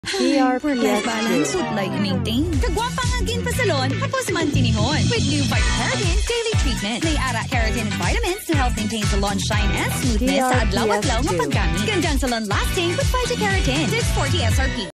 Vitakeratin Radio Ad 2H 2022 15s (Hiligaynon Version)
This is the Hiligaynon Version of the 2H 2022 Radio Ad for Unilever's own Expert Salon Treatment Brand - Vitakeratin.

DRPS Two 2437: Vitakeratin Radio Ad 2H 2022 15s (Hiligaynon Version)